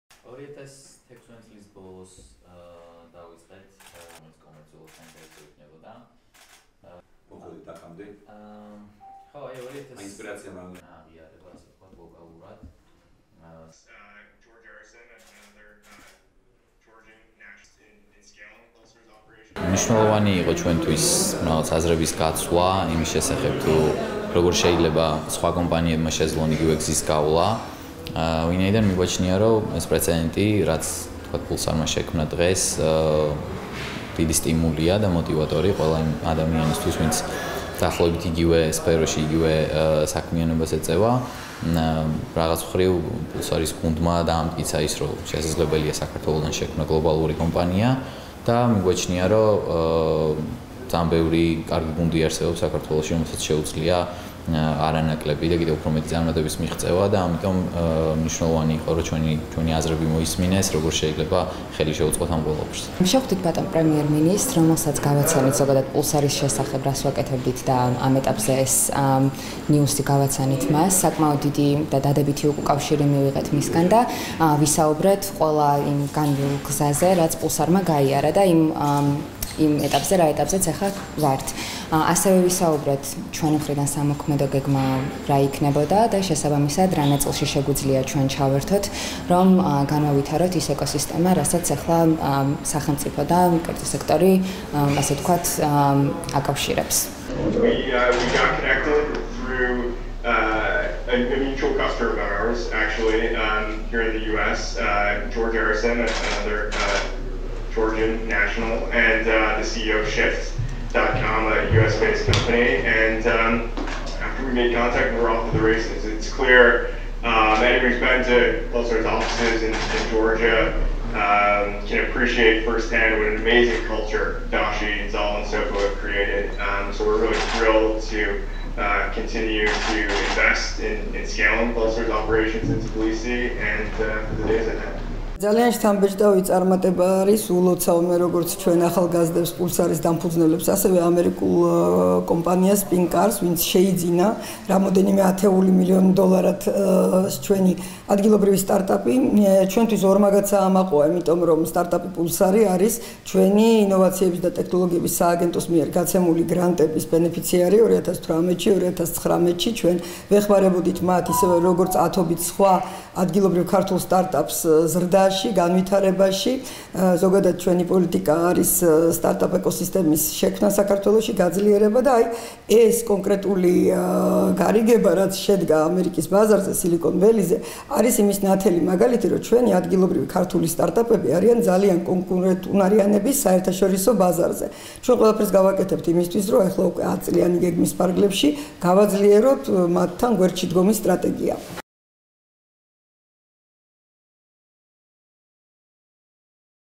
ირაკლი ღარიბაშვილმა სტარტაპ Pulsar AI-ის დამფუძნებლებს მიღწეული წარმატება მიულოცა
საქართველოს პრემიერ-მინისტრი ირაკლი ღარიბაშვილი, ქართული სტარტაპის, Pulsar AI-ის დამფუძნებლებს შეხვდა, რომელიც ავტოინდუსტრიაში ციფრული გაყიდვების სისტემების გლობალურმა ლიდერმა, ამერიკულმა კომპანია SpinCar-მა შეისყიდა.

შეხვედრაზე Pulsar AI-ის დამფუძნებლებმა ქართული სტარტაპის ისტორიასა და წარმატების გზაზე ისაუბრეს. ირაკლი ღარიბაშვილმა მათ ქართული ინოვაციური პროექტისთვის მადლობა გადაუხადა და წარმატებები უსურვა.

მთავრობის ადმინისტრაციაში გამართულ შეხვედრაზე აღინიშნა, რომ Pulsar AI-ის წარმატება ნათელი მაგალითია იმისა, რომ ადგილობრივი ქართული სტარტაპები ძალიან კონკურენტუნარიანები არიან საერთაშორისო ბაზარზე.

შეხვედრაზე აღინიშნა, რომ ის პრეცედენტი, რაც Pulsar AI-მა შექმნა, დიდი სტიმული და მოტივაციაა ყველა იმ ადამიანისთვის, ვინც აღნიშნულ სფეროში მოღვაწეობს.

შპს "პულსარი", ეკონომიკისა და მდგრადი განვითარების სამინისტროს ინოვაციების და ტექნოლოგიების სააგენტოს მიერ გაცემული გრანტების ბენეფიციარია. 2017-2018 წლებში მათ მოიპოვეს 5000-ლარიანი გრანტი პროტოტიპირებისთვის და საერთაშორისო ღონისძიებაში მონაწილეობისთვის, სადაც ინვესტორებს წარუდგინეს აღნიშნული პროტოტიპი. 2018 წელს, სტარტაპერებმა დამატებით სააგენტოს 100 000-ლარიან საგრანტო პროგრამაში გაიმარჯვეს, ხოლო 2020 წელს 650 000-ლარიან საგრანტო პროგრამაში. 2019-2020 წლებში „პულსარმა" 1.2 მილიონი აშშ დოლარის კერძო ინვესტიცია მოიზიდა აშშ-დან, სილიკონის ველიდან და საბოლოოდ, ქართული სტარტაპი რამდენიმე ათეულ მილიონ აშშ დოლარად გაიყიდა.

Pulsar AI მუშაობს კლიენტსა და ავტომობილის დილერს შორის სრულად ავტომატიზებული კომუნიკაციის შექმნაზე ხელოვნური ინტელექტის გამოყენებით. კომპანიის მიზანია, შექმნას ტექნოლოგია, რომელიც მისცემს საშუალებას ავტომობილების დილერებს სწრაფად და ეფექტურად მოემსახურონ პოტენციურ კლიენტებს ელექტრონული ფოსტის, მოკლე ტექსტური შეტყობინებისა თუ ვებ-ჩეთის საშუალებით.

საუბარი ასევე შეეხო საქართველოში ეკოსისტემის შექმნას, როგორც მთავრობის პრიორიტეტს. აღინიშნა, რომ მთავრობა, 10-წლიანი გეგმის ფარგლებში კიდევ უფრო მეტად გააძლიერებს მსგავსი ინოვაციური სტარტაპების მხარდაჭერის სტრატეგიას. პრემიერ-მინისტრის შეფასებით, ქართული ინოვაციური პროდუქტის პოზიციონირება გლობალურ ბაზარზე უაღრესად მნიშვნელოვანია.

მთავრობის ადმინისტრაციაში გამართულ შეხვედრას, საქართველოს ეკონომიკისა და მდგრადი განვითარების მინისტრი ნათია თურნავა, მინისტრის მოადგილე, ირაკლი ნადარეიშვილი, „საქართველოს ინოვაციების და ტექნოლოგიების სააგენტოს" ხელმძღვანელი ავთანდილ კასრაძე ესწრებოდა. შეხვედრას, ვიდეოკონფერენციის რეჟიმში, ამერიკის შეერთებული შტატებიდან შეუერთდა ციფრული პლატფორმის, SpinCar-ის თანადამფუძნებელი მაიკლ ქვიგლი.